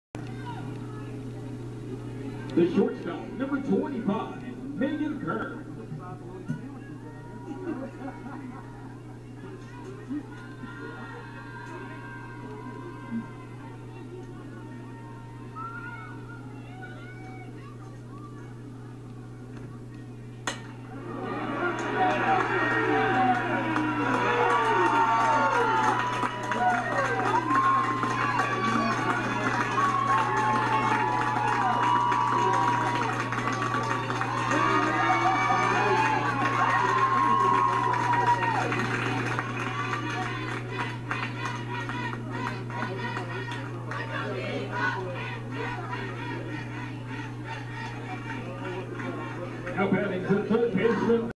Villanova softball's Megan Kern hits 2 run home run vs. Marshall in Georgia Tech tourney
Villanova softball's Megan Kern hits this two run homer vs. Marshall in the first inning during the Georgia Tech softball tournament, 2/11/22.
This video belongs to the Atlantic Coast Conference and the NCAA.